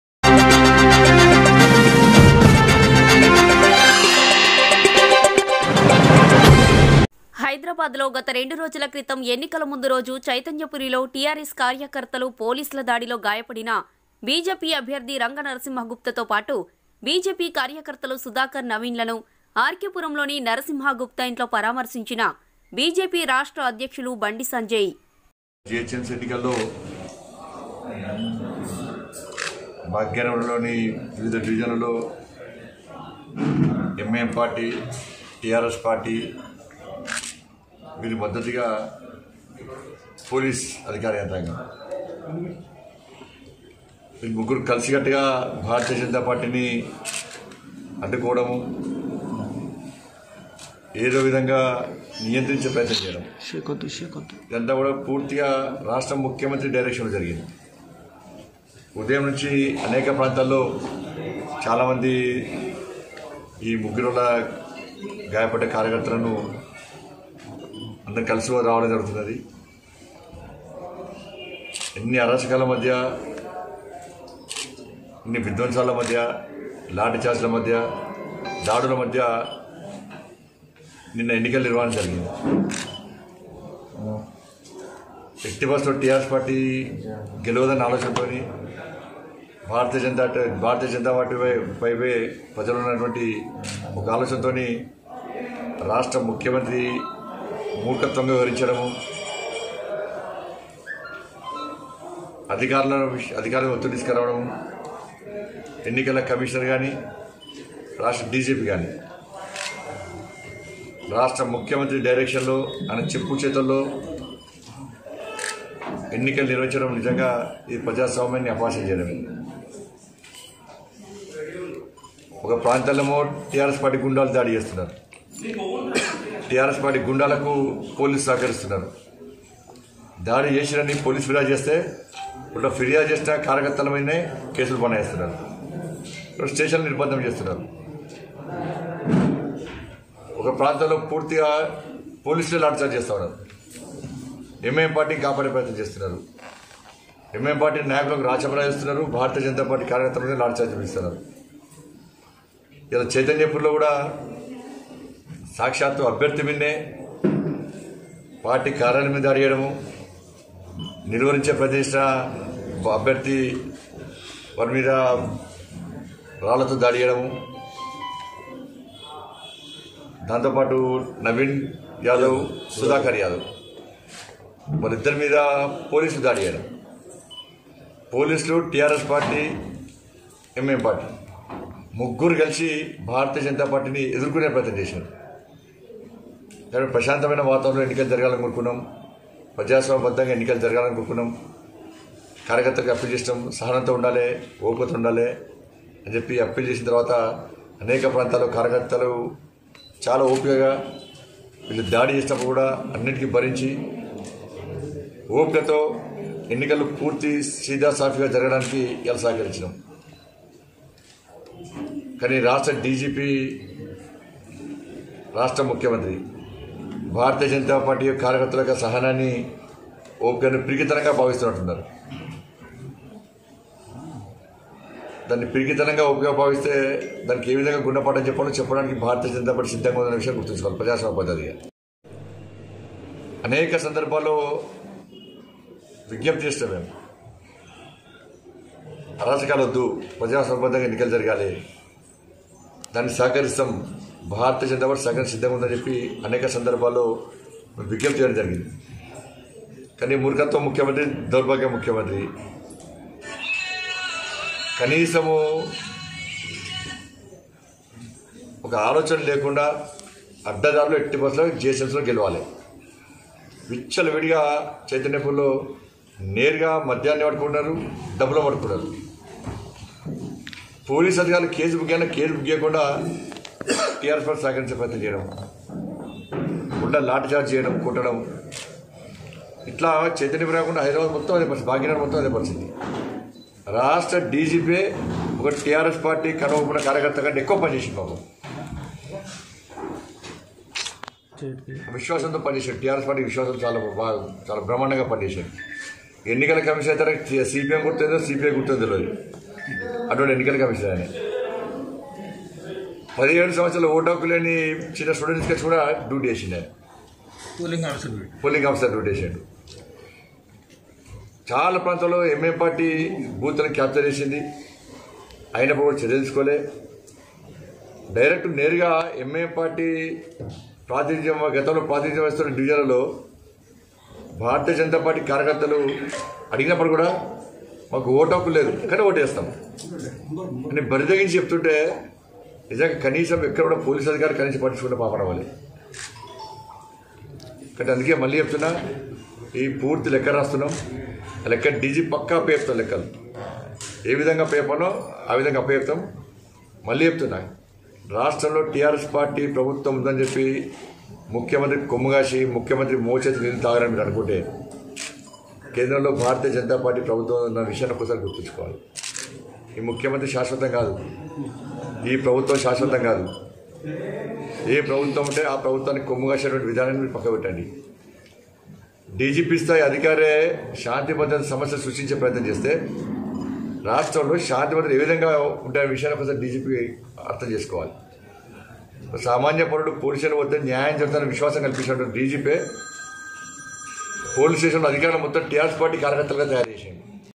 పోలీసుల దాడిలో గాయపడిన బీజేపీ అభ్యర్థులను పరామర్శించిన బీజేపీ రాష్ట్ర అధ్యక్షులు బండి సంజయ్.
హైదరాబాద్ లో గత రెండు రోజుల క్రితం ఎన్నికల ముందు రోజు చైతన్యపురిలో టిఆర్ఎస్ కార్యకర్తలు,పోలీసుల దాడిలో గాయపడిన బీజేపీ అభ్యర్థి రంగ నర్సింహ గుప్తతో పాటు బీజేపీ కార్యకర్తలు సుధాకర్,నవీన్ లను ఆర్ కె పురంలోని నర్సింహ గుప్త ఇంట్లో పరామర్శించిన బీజేపీ రాష్ట్ర అధ్యక్షులు బండి సంజయ్.